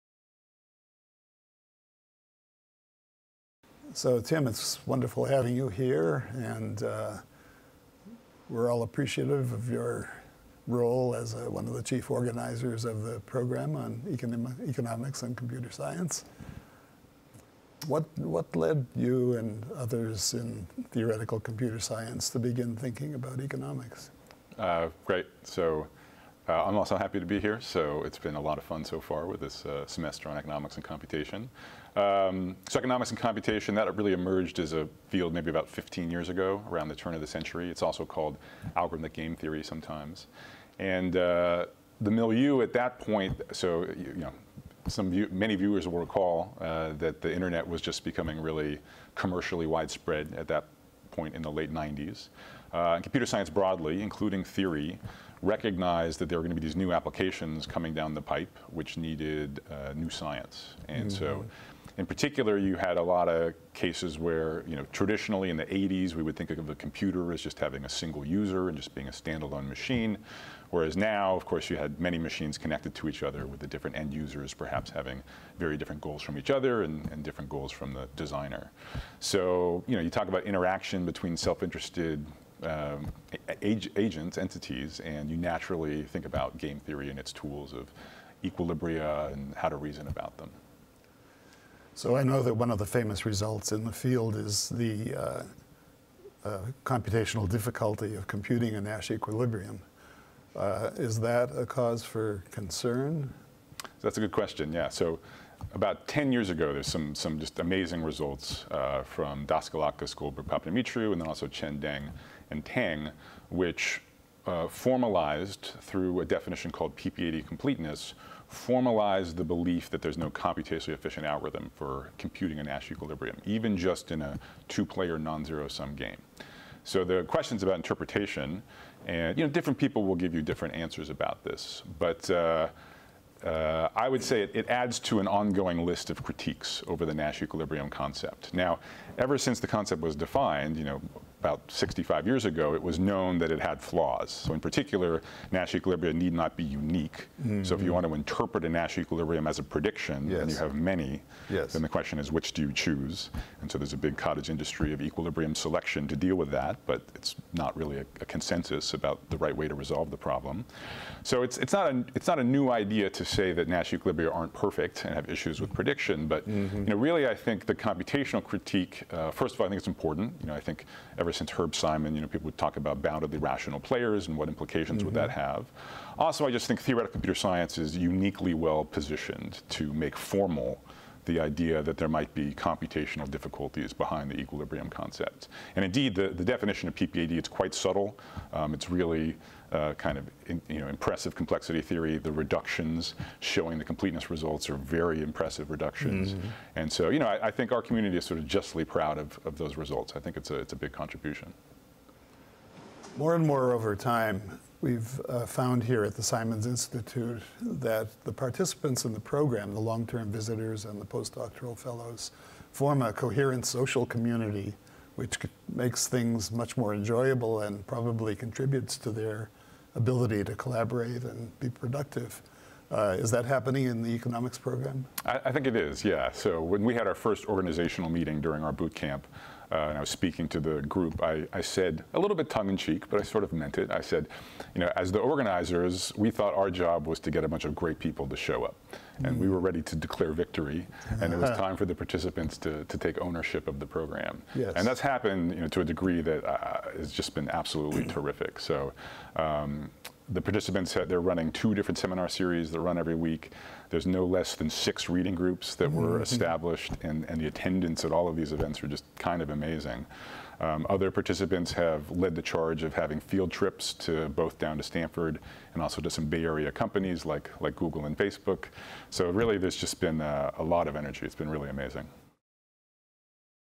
From the Inside: Economics and Computation
Richard Karp sat down with Tim Roughgarden to discuss the Fall 2015 program on Economics and Computation.